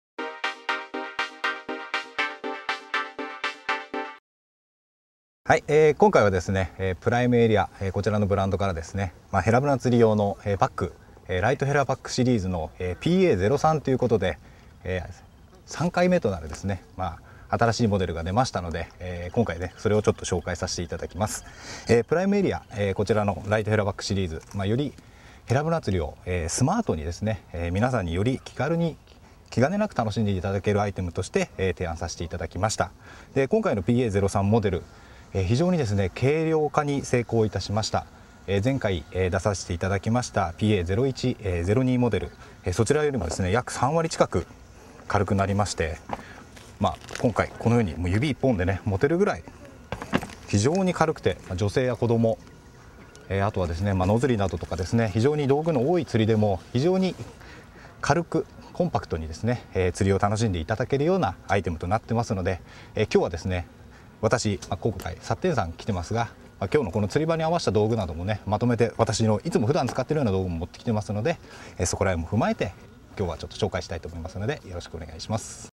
MARUKYU 라이트헤라백 PA-03 세트 Yuichi Toida
이번에 프라임 에리어 브랜드로 발매해왔던 라이트헤라백 시리즈의 제3탄인「라이트헤라백 PA-03」을 소개합니다.